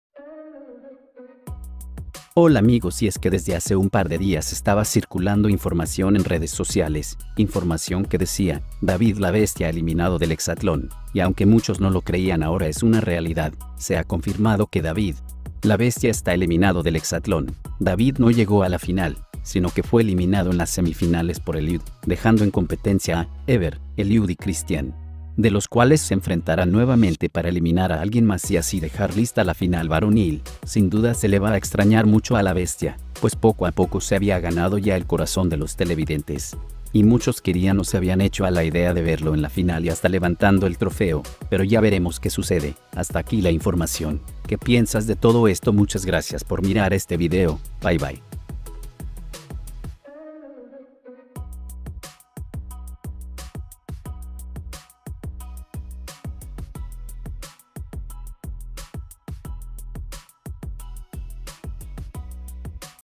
David no pudo llegar a la final "Queda fuera de inmediato"
Hola amigos desde hace un par se días estaba circulando información en redes sociales, información que decía: David la bestia eliminado del Exatlon, y aunque muchos no lo creían ahora es una realidad